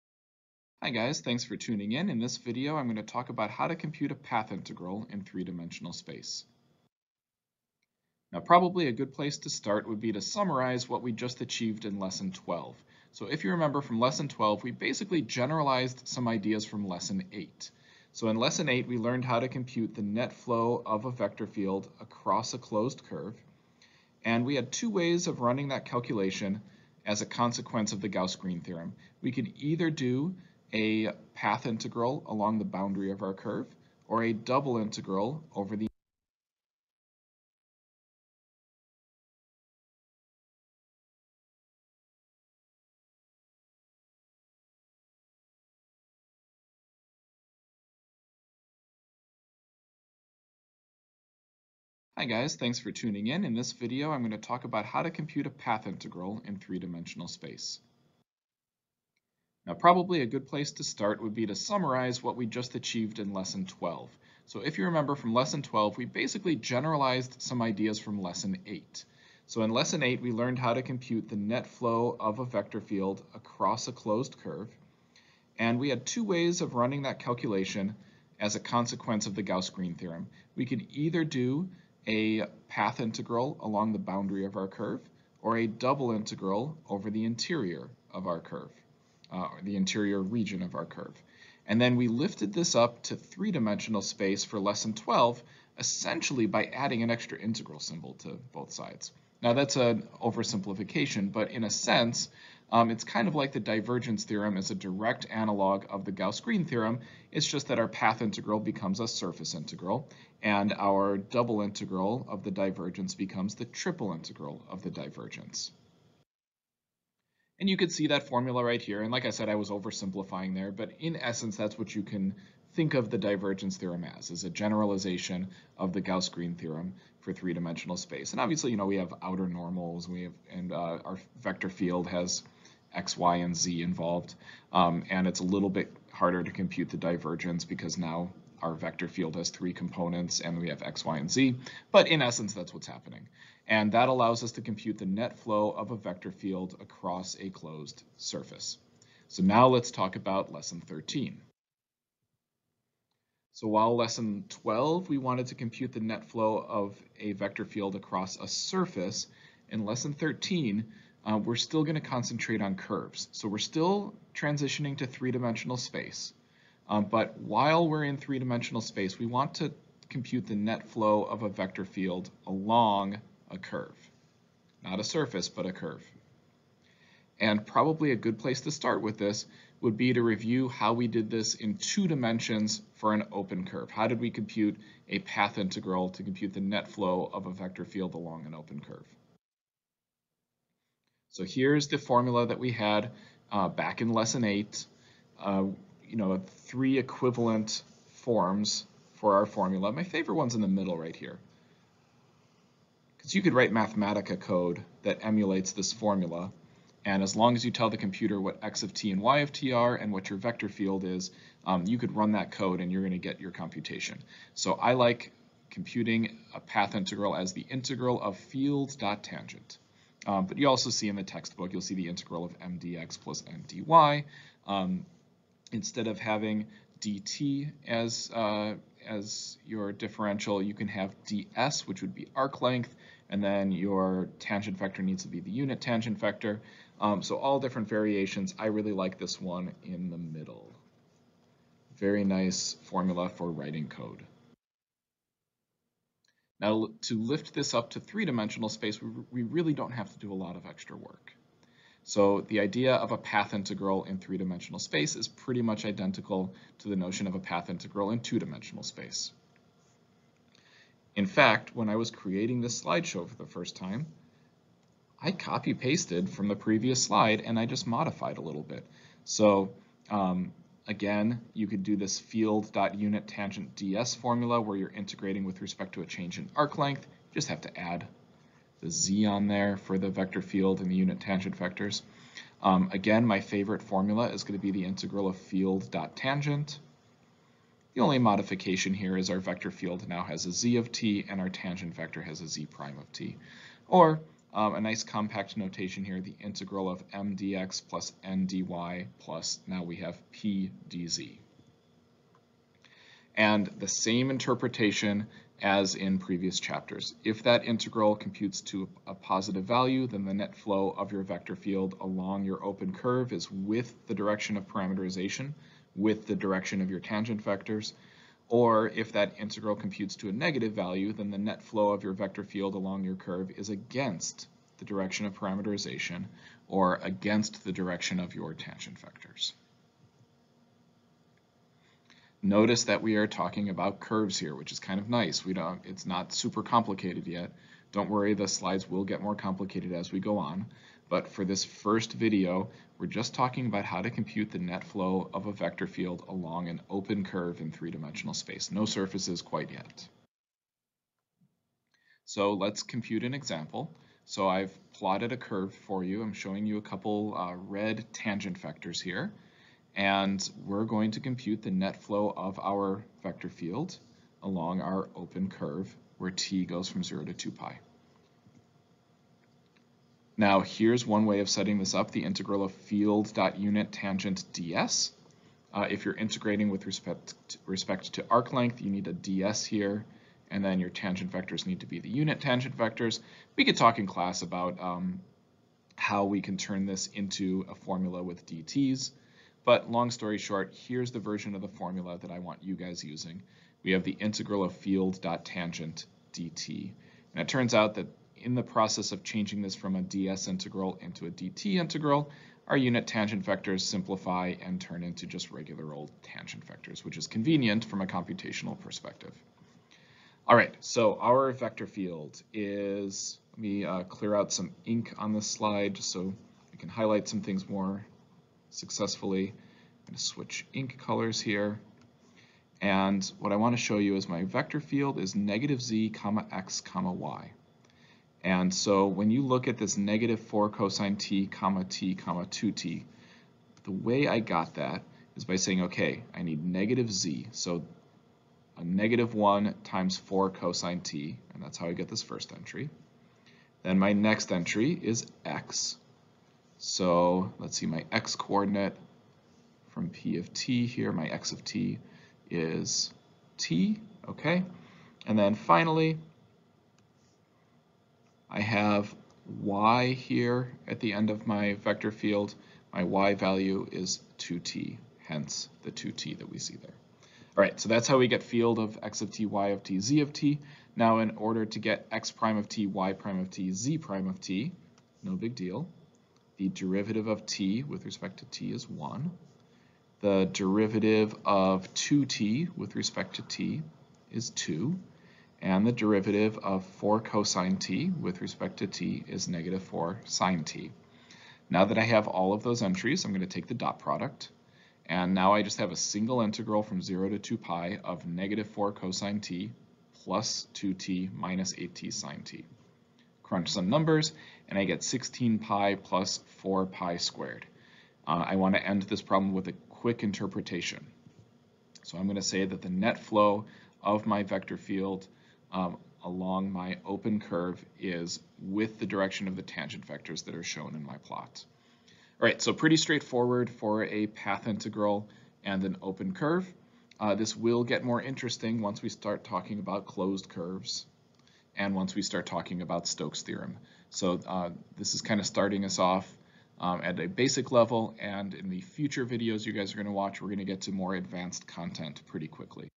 Path Integrals in 3D (Lesson 13, Part 1)
In this video, we demonstrate how to compute a path integral in 3D space.